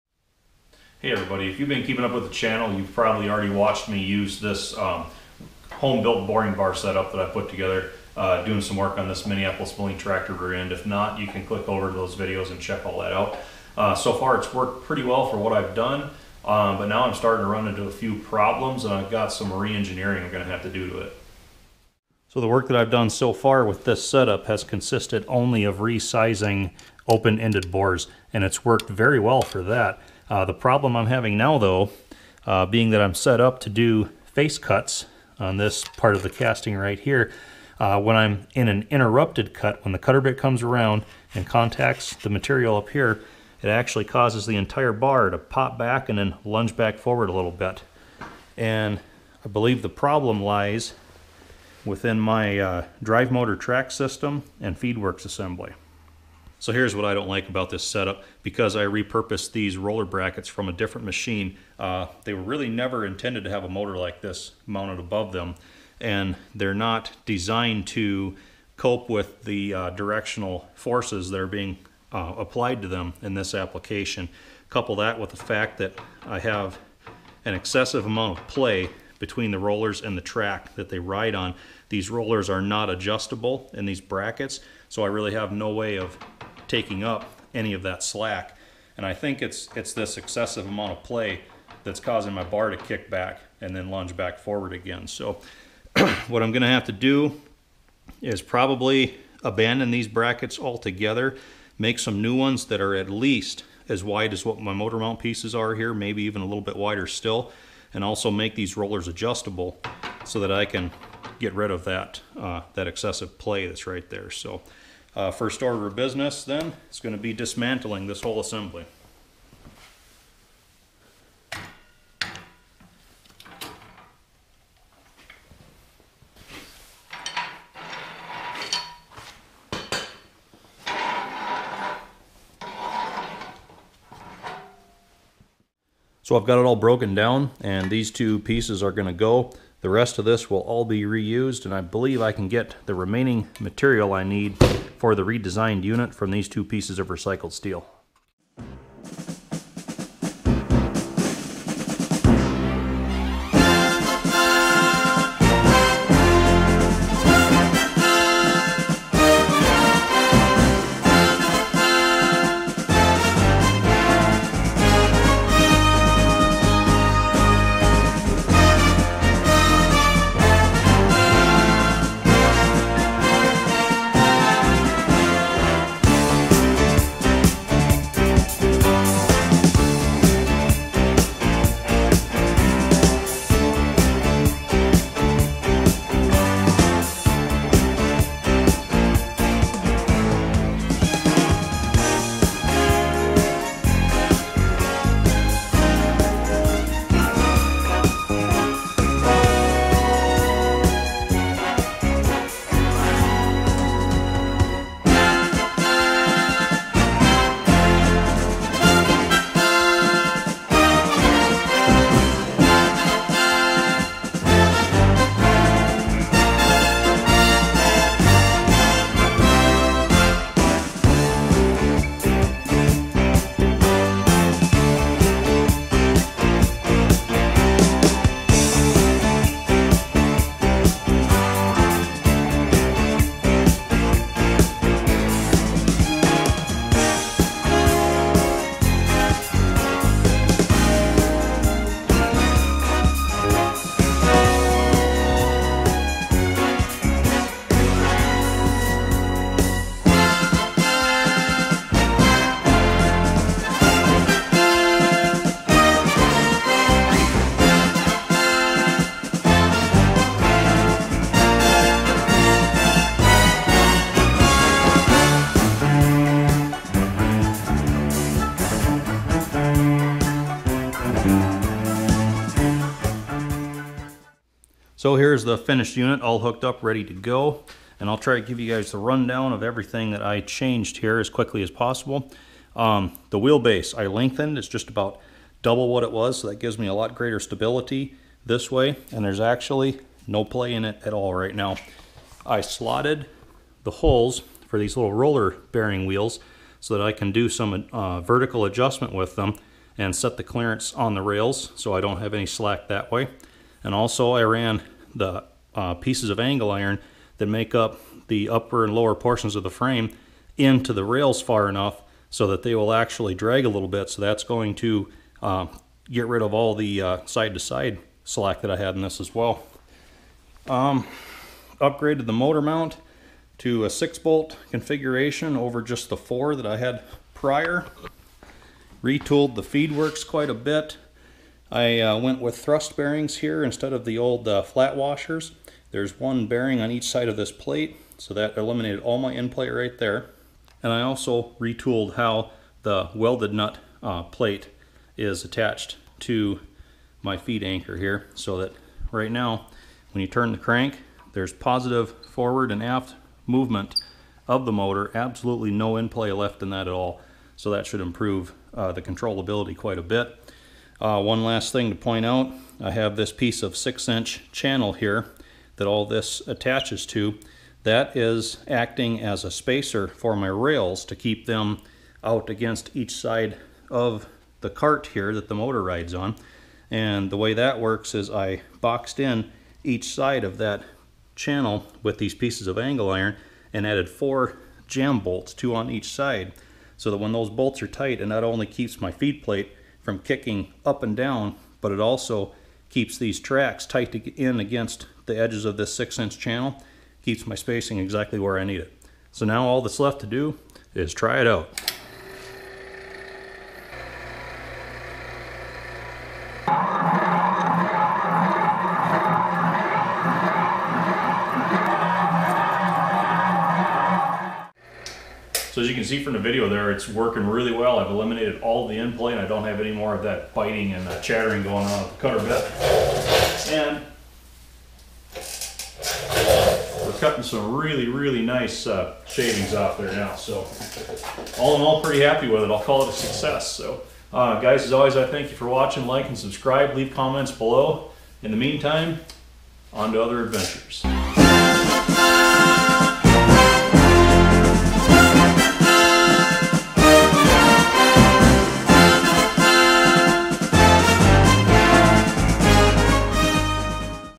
Boring Bar Feed Works Upgrade/Redesign
Redesigning the feed works mechanism on my home-built boring bar setup, after running into some problems with the old setup.